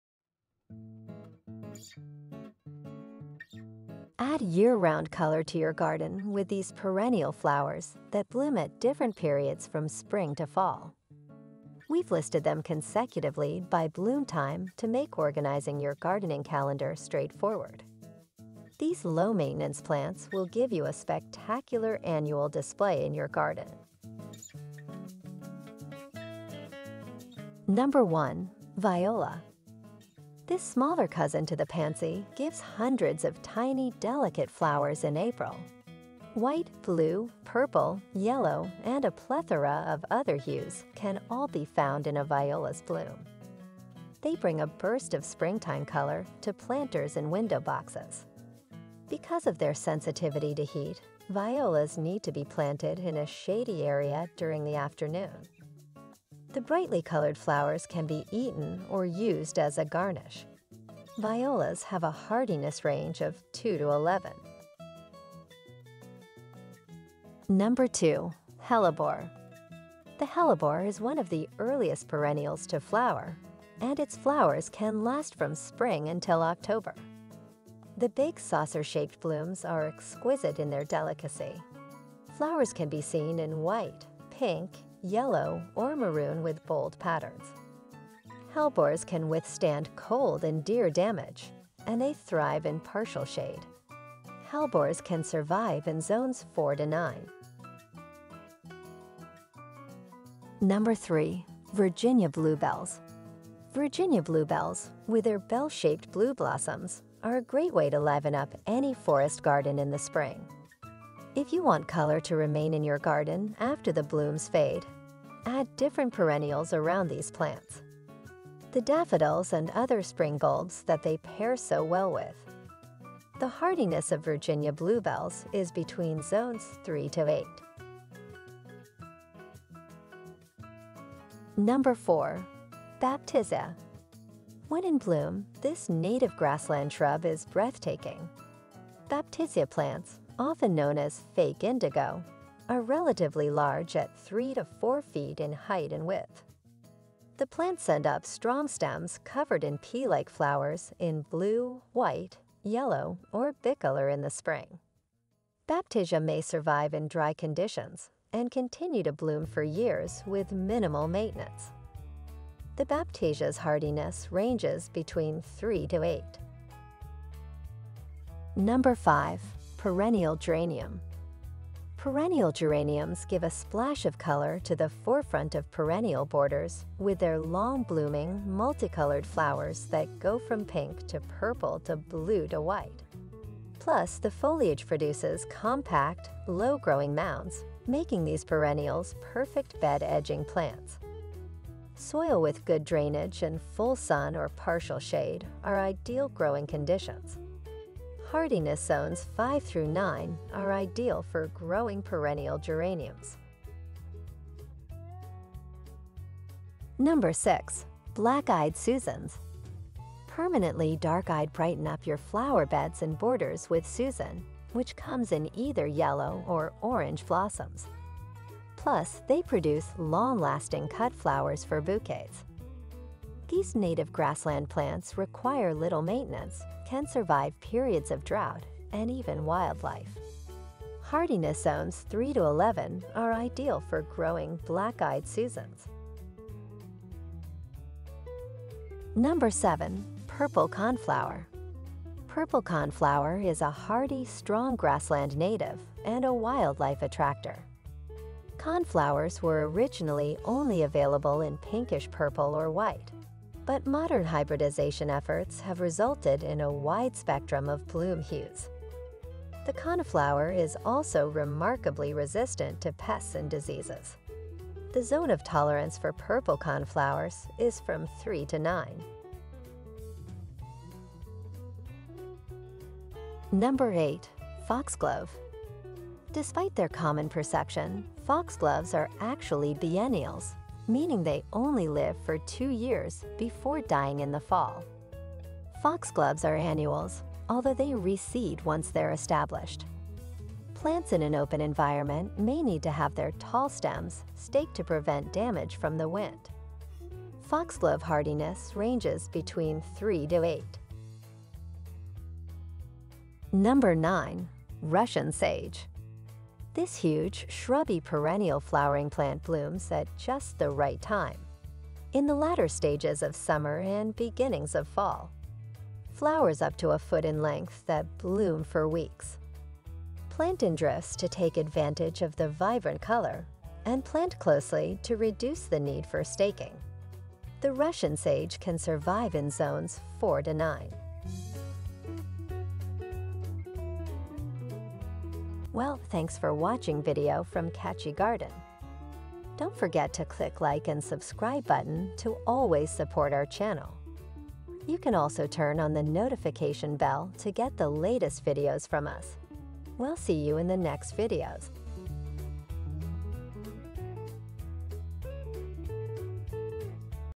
9 Best Perennial Flowers Are a Riot of Color All Year Long 🌻🌸👍 // Gardening Ideas
Add year-round color to your garden with these perennial flowers that bloom at different periods from spring to fall.

We've listed them consecutively by bloom time to make organizing your gardening calendar straightforward.

These low-maintenance plants will give you a spectacular annual display in your garden.
⏱  9 Best Perennial Flowers Are a Riot of Color All Year Long
00:00 Intro
00:26 Viola
01:05 Hellebore
01:39 Virginia Bluebells
02:10 Baptisia
02:47 Perennial Geranium
03:25 Black-eyed Susans
03:58 Purple coneflower
04:23 Foxglove
05:02 Russian Sage
05:35 Outro

Long Blooming Flowering Perennials,Foundation Plants,Border Plants